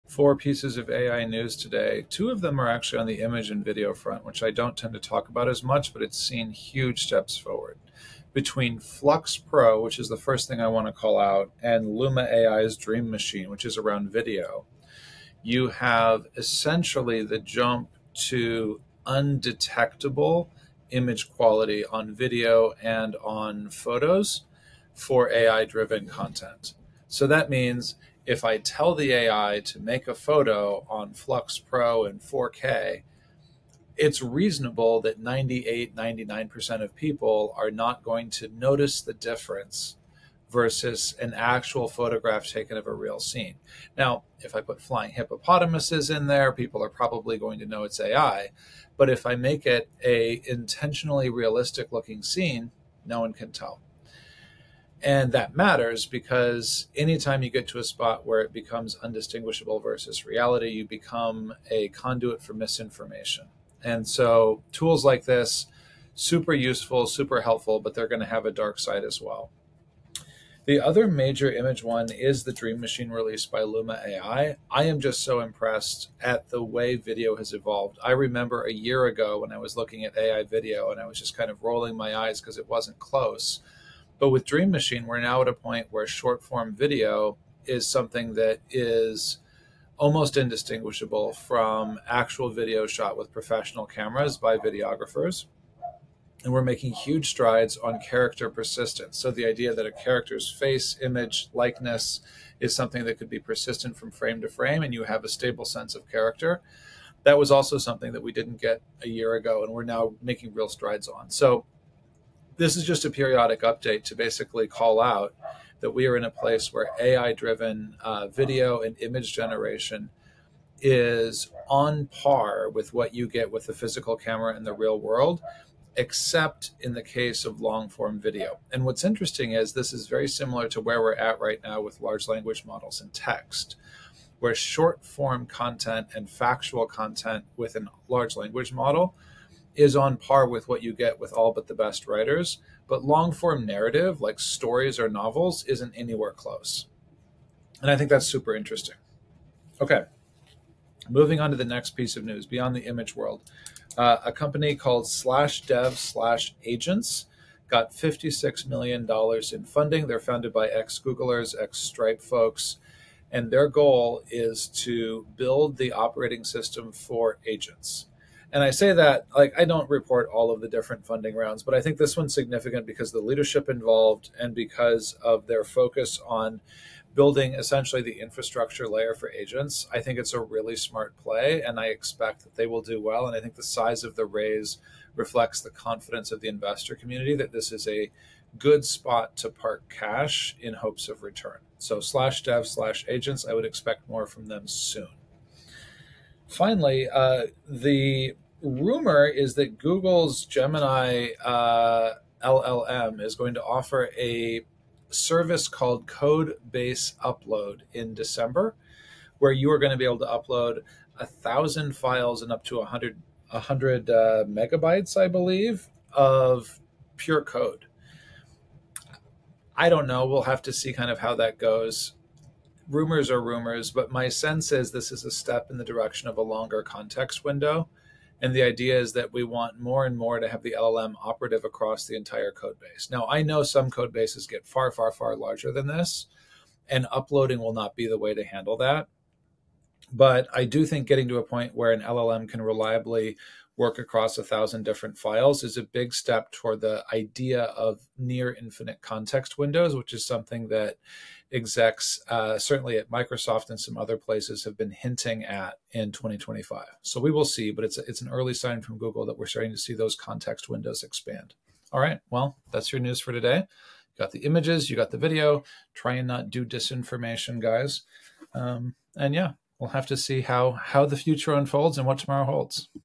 AI News: $56M for AI Agent OS, AI Generated Ad Videos, Upload your Codebase in Google Gemini
Looks like Google Gemini codebase upload did drop!

Takeaways:
 1. Flux Pro’s Realism in 4K: Flux Pro can generate photos so realistic that 98–99% of people can’t distinguish them from real images, marking a major leap for AI content creation.
 2. Dream Machine Revolutionizes Video: Luma AI’s Dream Machine delivers short-form videos that rival professional videography, with significant advancements in character persistence and frame stability.
 3. Potential for Misinformation: With AI-generated images and videos becoming indistinguishable from reality, these tools have incredible potential but also pose serious risks for misuse, such as disinformation.
 4. Agent Infrastructure Advances: Slash Dev Slash Agents raised $56M to build the “operating system for agents,” reflecting the rising importance of AI-powered workflows in business.
 5. Gemini’s Codebase Upload Feature: Google’s Gemini LLM may soon allow users to upload 1,000 files (up to 100MB), an important step toward expanded context windows and improved coding assistance.
 6. Near-Infinite Context Windows by 2025: Execs at major tech companies, including Microsoft, hint at near-infinite AI context windows on the horizon, enabling more comprehensive large-scale processing.

Quotes:
“We’re at a point where AI-generated visuals rival reality in quality, but the dark side of misinformation can’t be ignored.”
“Agent infrastructure is the next frontier for AI—expect breakthroughs in workflows and integration.”
“Google Gemini’s rumored codebase upload feature signals the race toward limitless AI context processing.”

Summary:
Flux Pro and Luma AI’s Dream Machine are reshaping AI-generated content, bringing photo and video quality to near-perfect realism. While these tools inspire creativity, they also raise concerns about misinformation. Slash Dev Slash Agents made waves with a $56M funding round to develop agent infrastructure, and Google’s Gemini LLM is rumored to debut codebase upload capabilities. These innovations underscore the fast-evolving AI landscape, hinting at near-infinite AI context windows by 2025. With these developments, we’re closer than ever to AI transforming workflows, creativity, and productivity on a global scale.